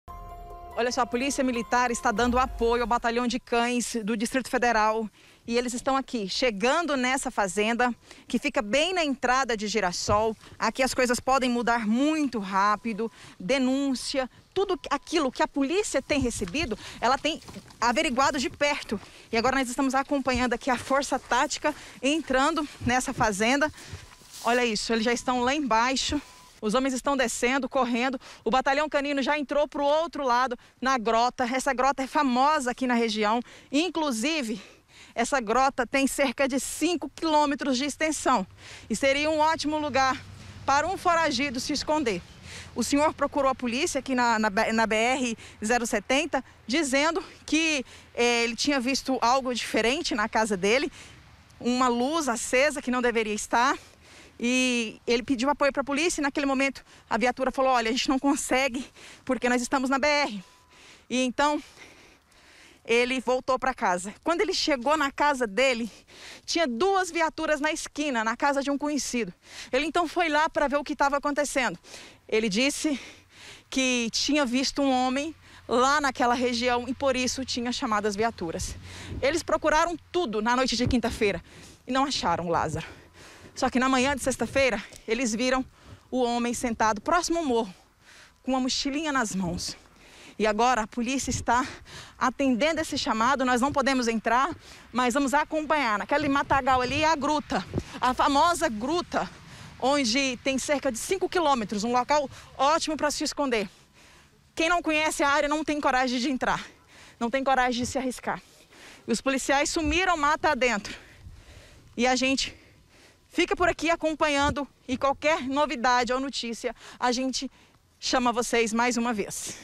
URGENTE: POLICIAIS LUTAM PARA ATENDER CHAMADO DE DENÚNCIA CASEIRO DE CHÁCARA DISSE TER VISTO LÁZARO
URGENTE! NOSSA EQUIPE RECEBEU INFORMAÇÕES DE QUE UM CASEIRO DE UMA CHÁCARA TERIA VISTO UM HOMEM PRÓXIMO À SEDE DA PROPRIEDADE. ELE ACIONOU A POLÍCIA MILITAR E NOSSA EQUIPE ACOMPANHA DE PERTO.